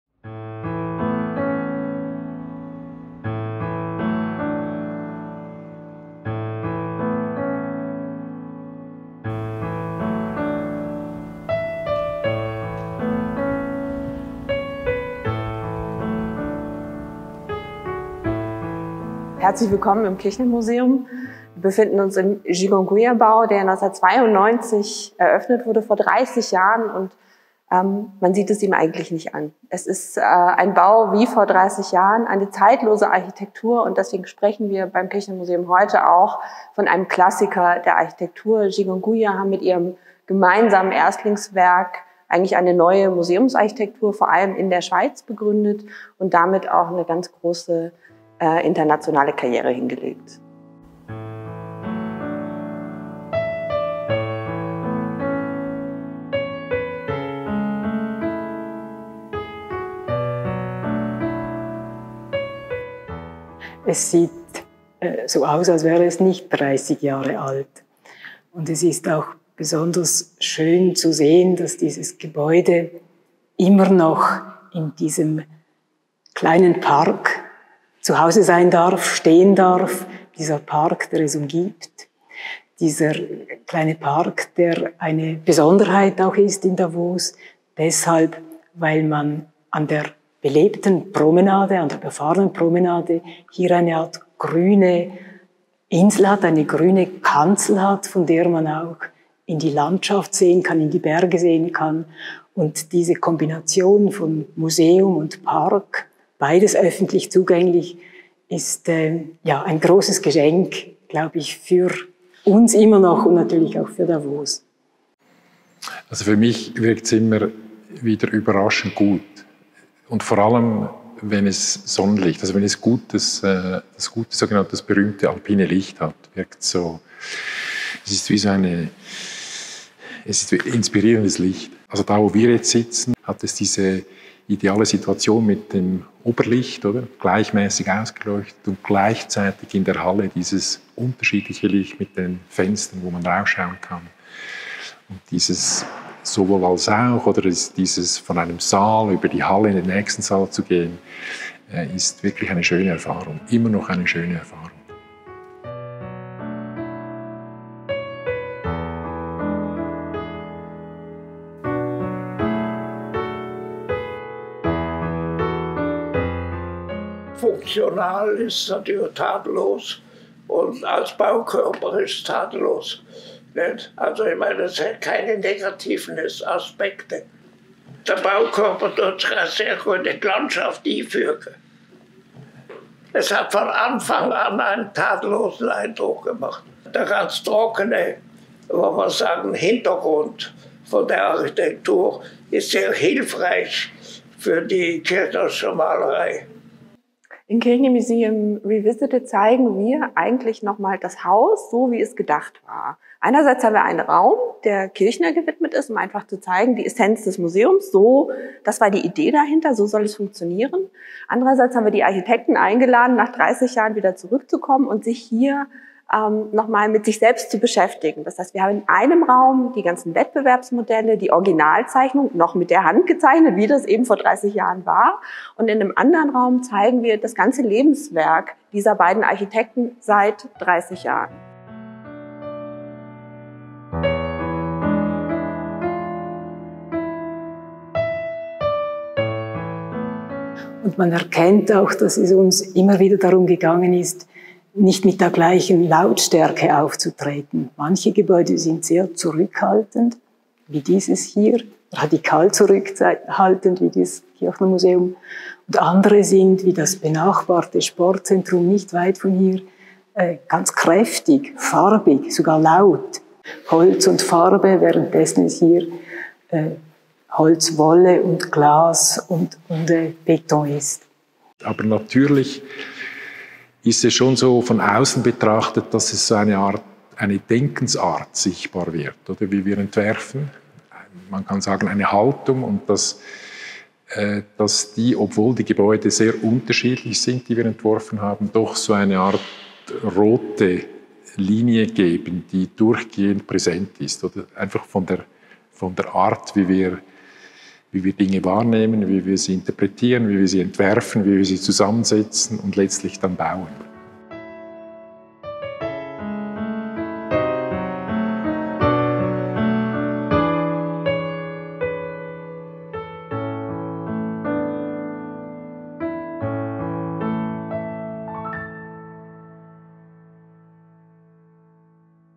Gigon/Guyer und das Kirchner Museum: Entstehungsgeschichte einer architektonischen Ikone
Wer hat das Kirchner Museum ins Leben gerufen? Was bietet es den Museumsbesucher:innen, was den Davoser:innen, was den Touristen? Was leistet das Gebäude für die Kunst Ernst Ludwig Kirchners, was für die Werke anderer Kunstschaffender? Wie funktioniert das Museum – wie sieht es über, hinter und unter den Ausstellungssälen aus? Würde man es heute anders bauen? Die Ausstellung bietet vielschichtige Zugänge zur zeitlosen Architektur.